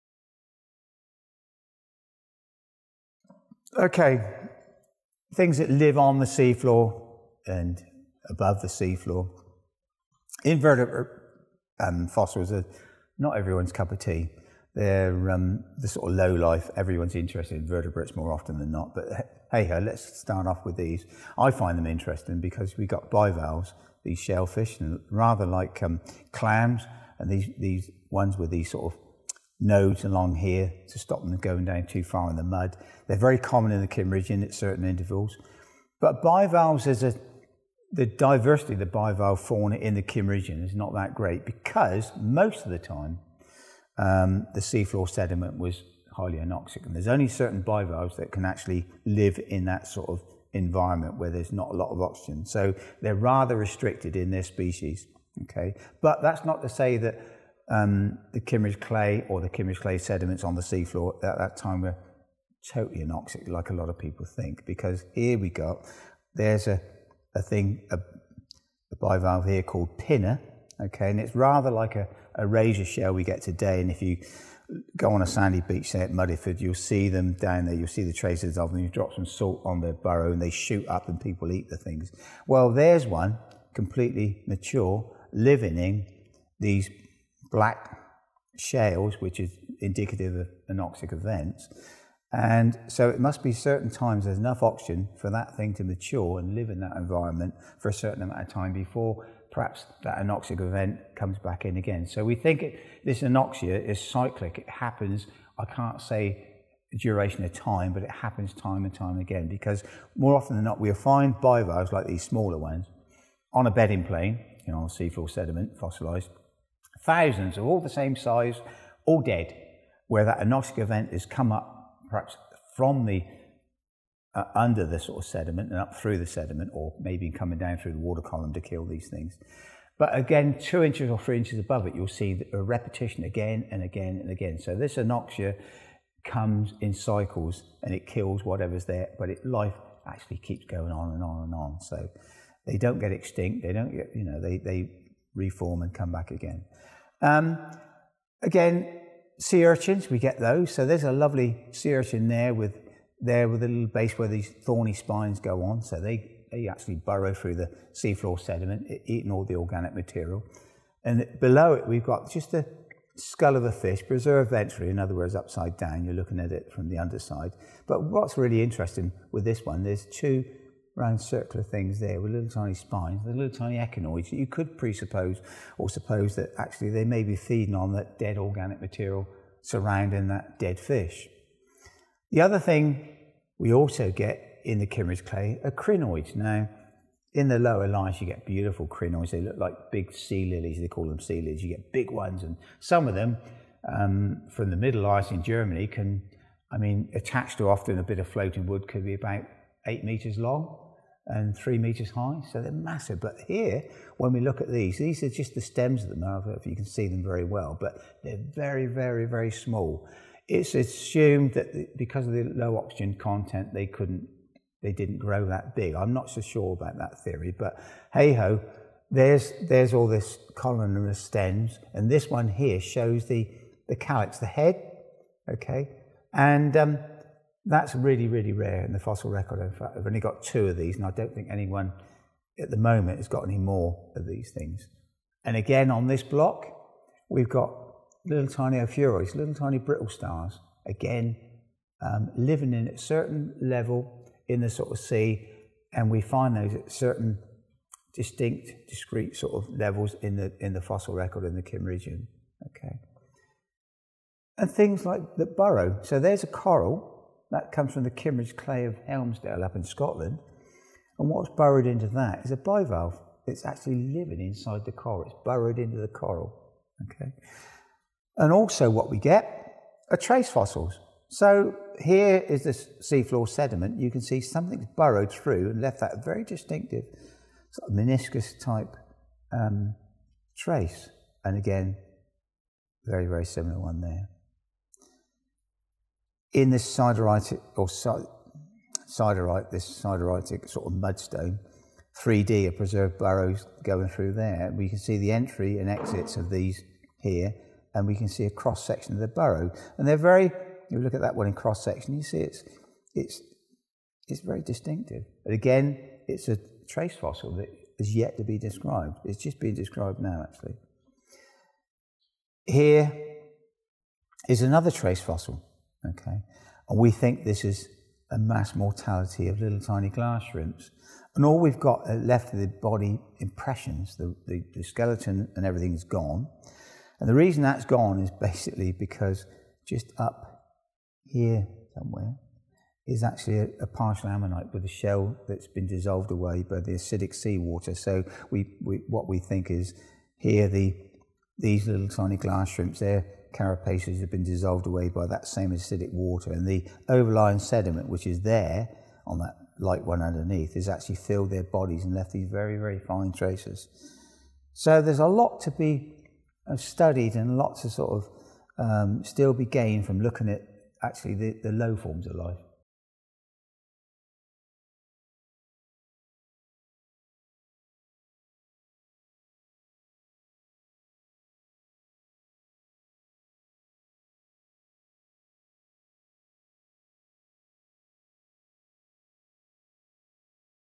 THE KIMMERIDGIAN SEA FLOOR | INVERTEBRATE FOSSILS | STEVE ETCHES
This week Steve Etches is back discussing some of his collection and in particular, prehistoric life on the sea floor during the Kimmeridgian time period.

► EQUIPMENT
Adobe AfterEffects
Adobe Premiere Pro
Adobe Audition
DaVinci Resolve

Executive Producer: Steve Etches
Producer: Ash Hall
Editor: Ash Hall